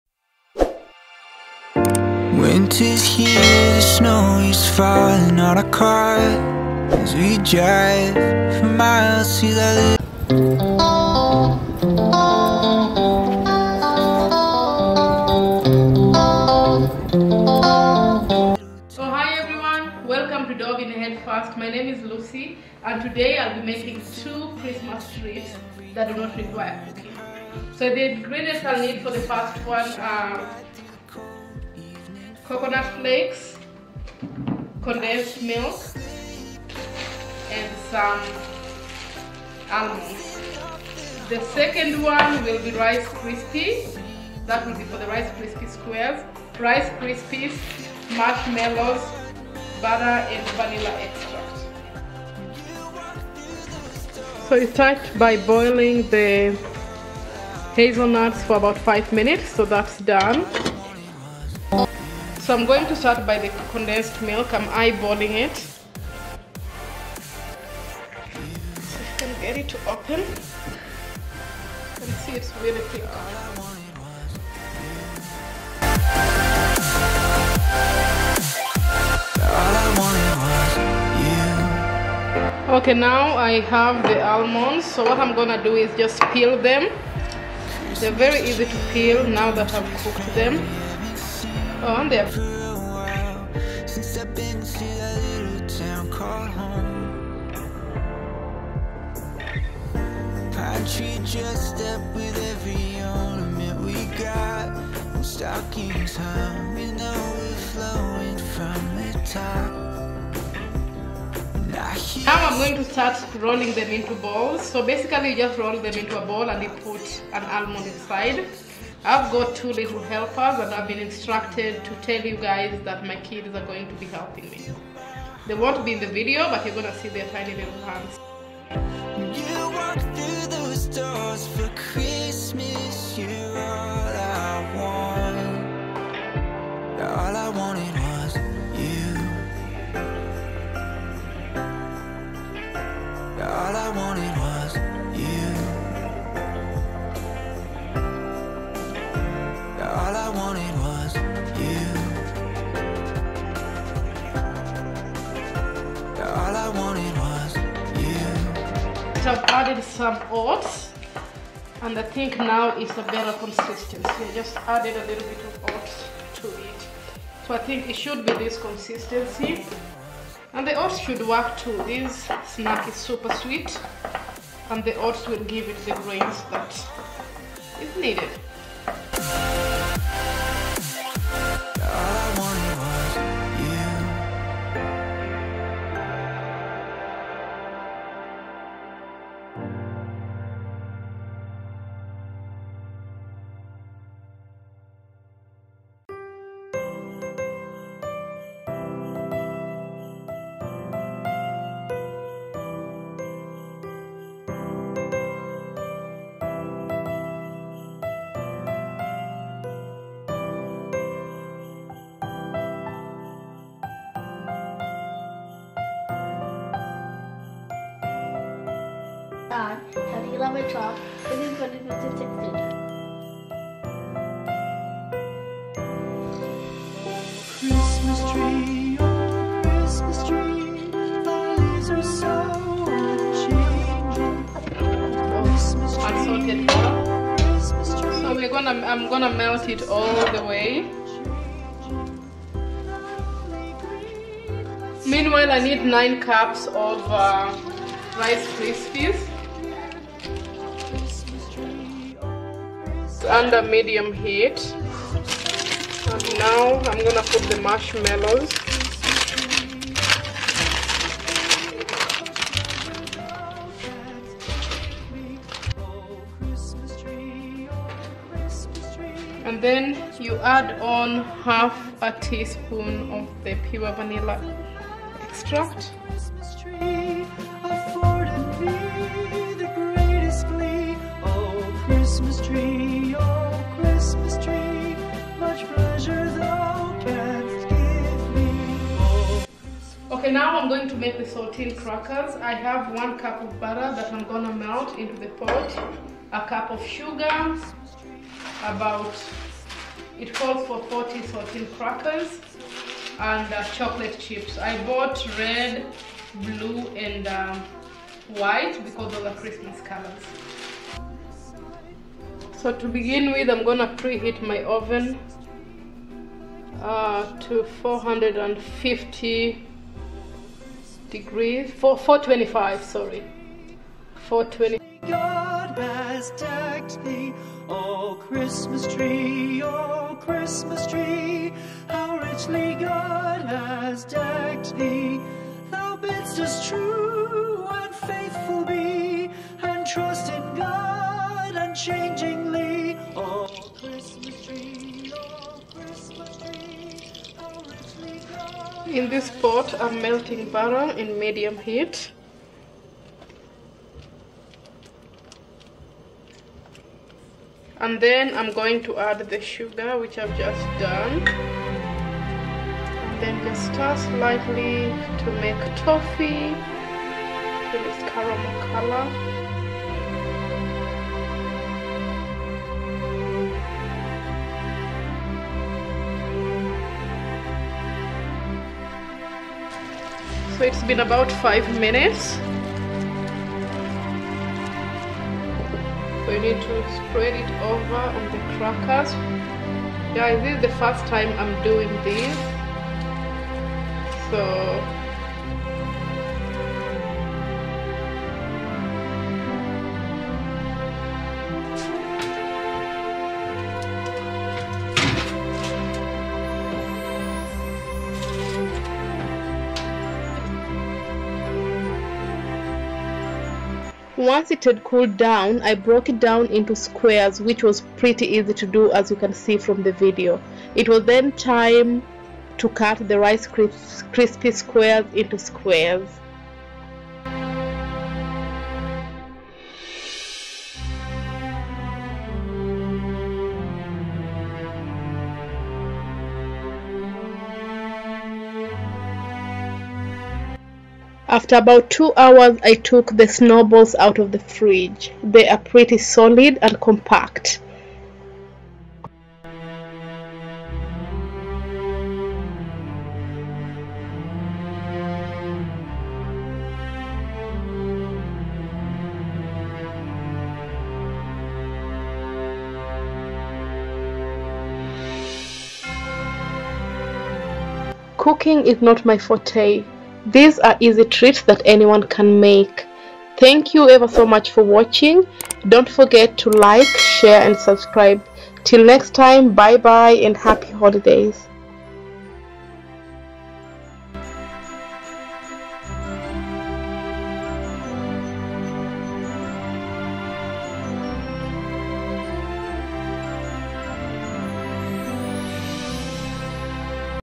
3 Easy Christmas treats || Festive season comfort food || Stress free, fool proof and quick.
Hello Everyone,
Super happy to share these 3 quick recipes, cooking is not my forte yet I managed to whip these up in about 2 hours. My family love it try these during this holiday season.

Recipes
1. Snow balls

2 and 3/4 cup of desiccated coconut  (250 g)
1 can of condensed milk (395 g)
1/4 cup of almonds (40 g)

2. Rice Krispie Treat

3. Saltine Toffee